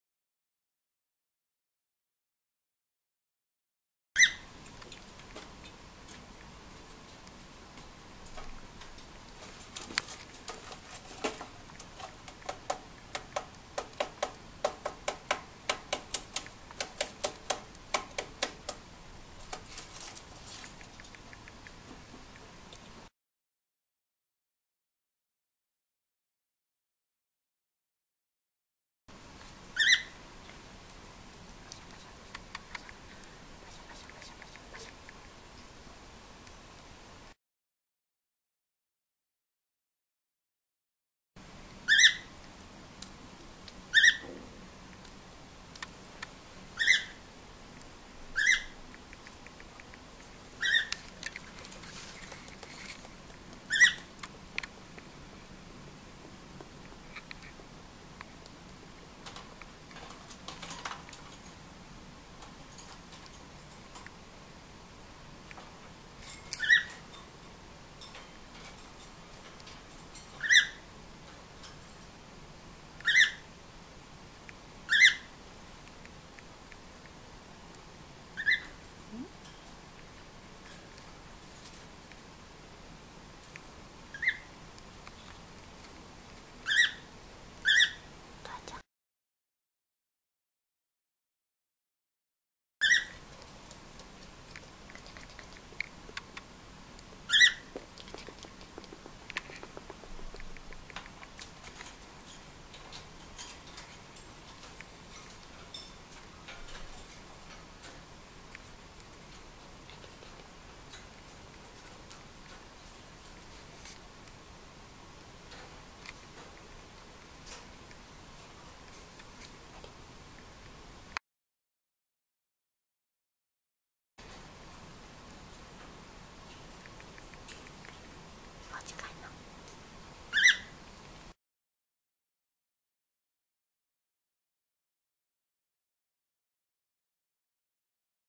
20180302_とらちゃん
オカメインコのとらちゃん、飛ぶ練習の風景です。